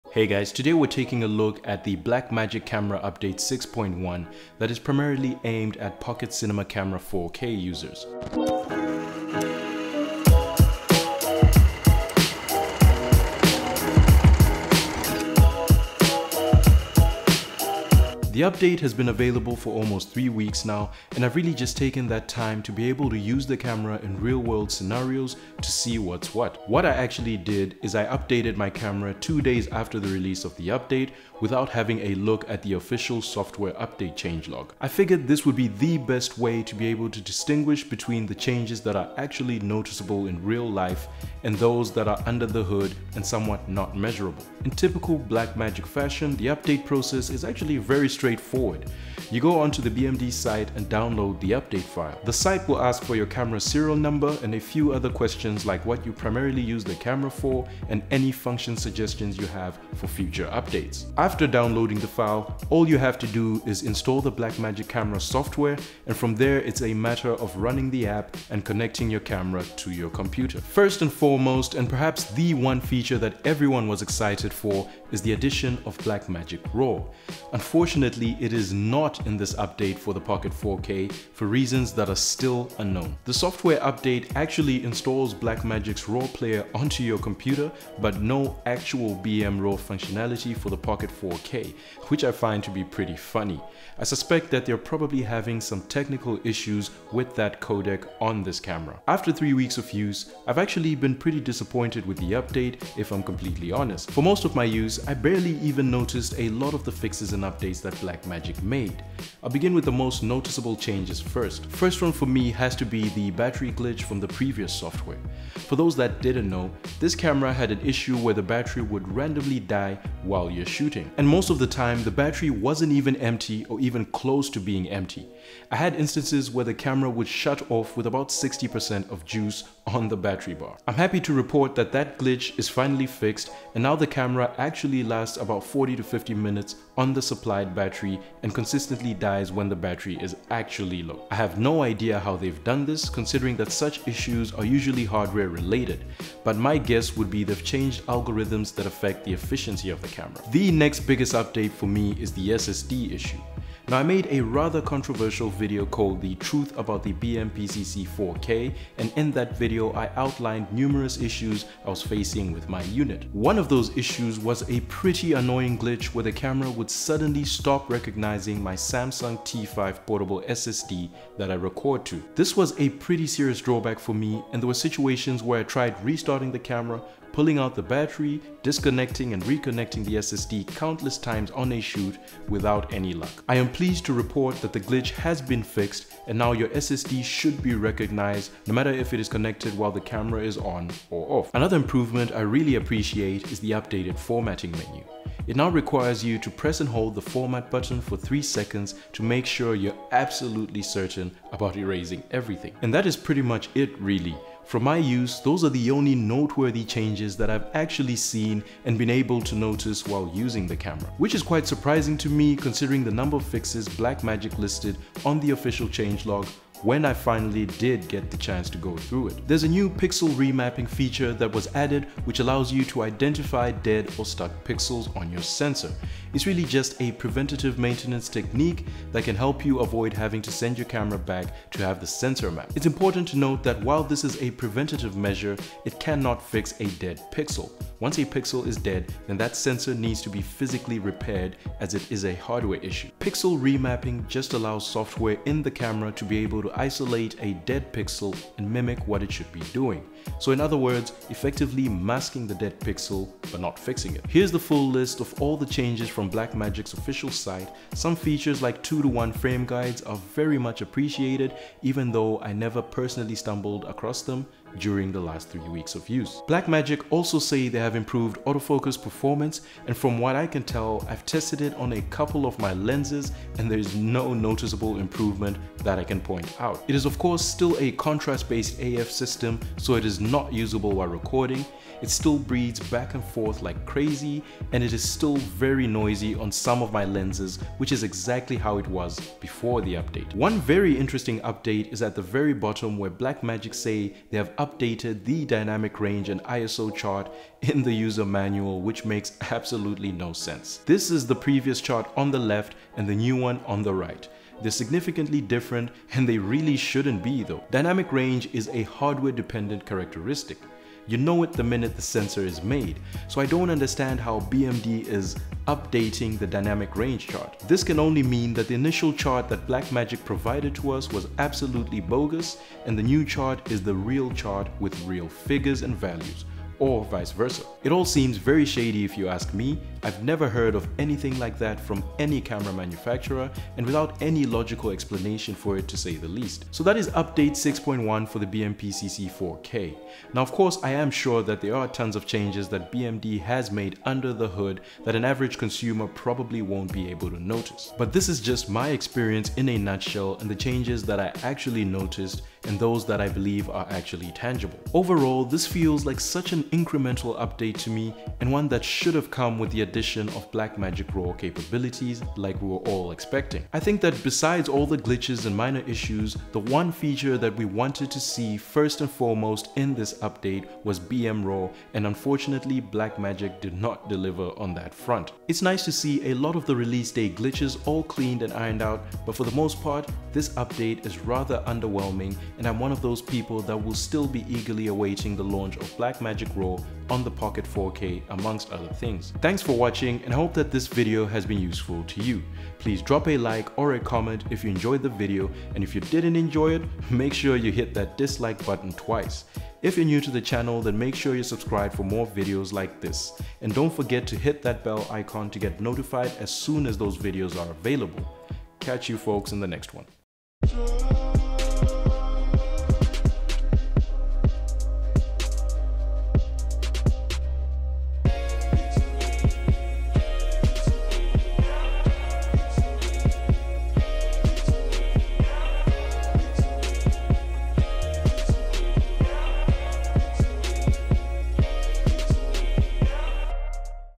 BMPCC 4K Firmware Update 6.1: Where is Blackmagic RAW?
In this video, we take a look at the new camera firmware update 6.1 from Blackmagic Design plus the features and improvements it brings to the table.